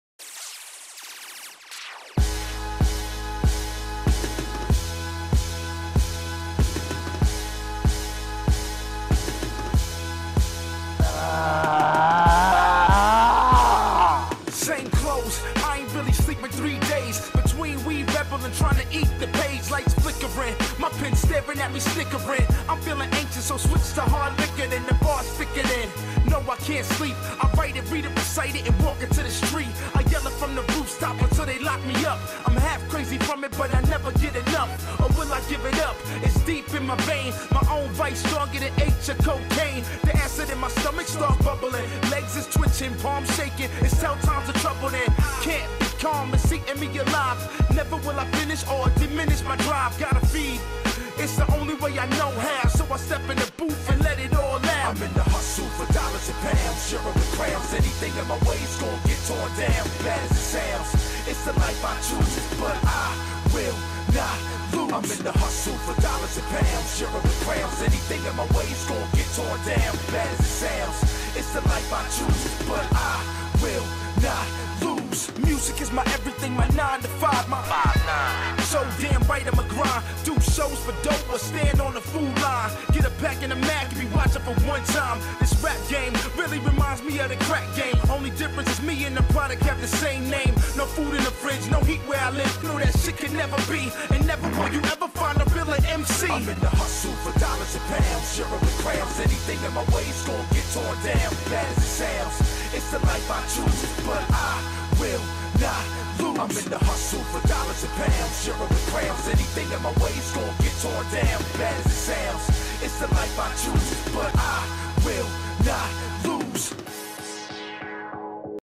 Monster event videoreportáž
Monster energy drink na koncertě hvězd českého rapu. Autor: Ondřej Svatoš,